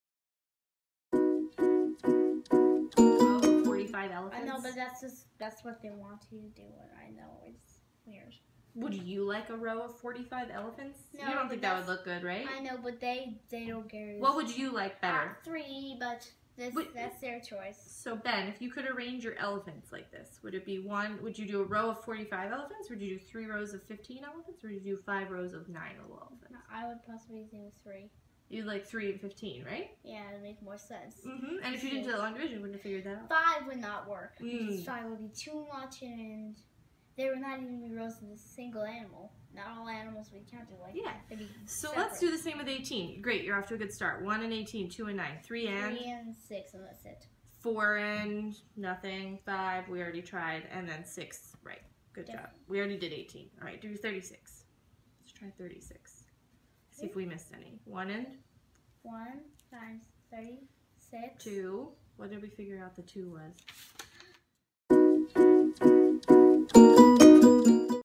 4th Grade Math Tutoring Session
My son's 4th grade math tutoring session.